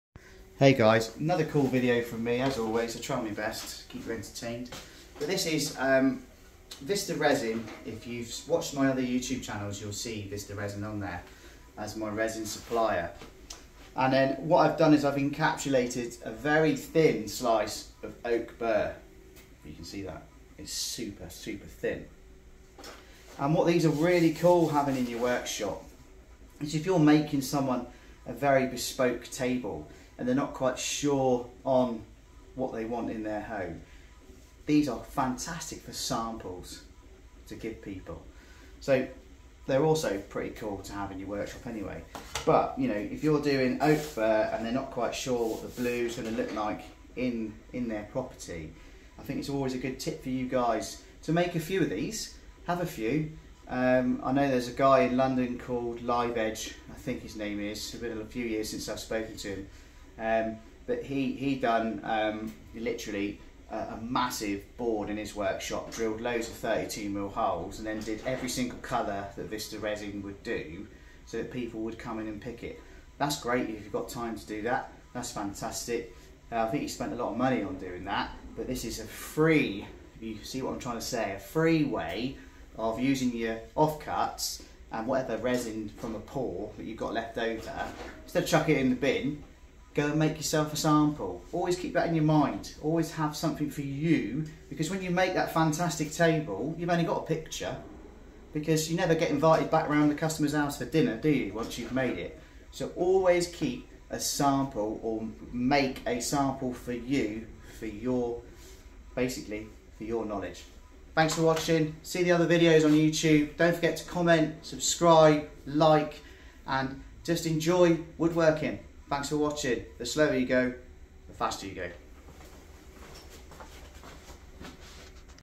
"Creative Crafting: Transforming Off Cuts into Unique Samples"
In this exciting video, we dive into the world of creative crafting by showing you a fantastic technique: transforming off cuts into unique samples. Join us as we explore the endless possibilities of repurposing those leftover materials. In this tutorial, we'll share step-by-step guidance on how to breathe new life into your off cuts and make them shine. Watch as we unlock the potential of these discarded pieces and showcase the artistry that can be achieved through resourcefulness. By turning your off cuts into stunning samples, you open up a treasure trove of opportunities for various projects. The sense of satisfaction and accomplishment that comes from salvaging these seemingly insignificant remnants is truly rewarding. Throughout the video, our expert craftsmen will walk you through a range of techniques, from clever cuts and artistic arrangements to innovative ways of displaying your creations. Witness the magic unfold as we guide you towards creating remarkable pieces that will captivate any audience. Not only will this tutorial inspire you to think outside the box, but it will also equip you with valuable skills that can be applied to future endeavors. Say goodbye to wasted materials and hello to a whole new world of creativity and resourcefulness. So, whether you're an experienced crafter seeking fresh inspiration or a beginner eager to discover new techniques, this video is perfect for you. Join us on this exploration of transforming off cuts into exceptional samples and get ready to embark on a journey of imagination and ingenuity. Don't miss out on this opportunity to tap into your creative potential and take your crafting skills to the next level.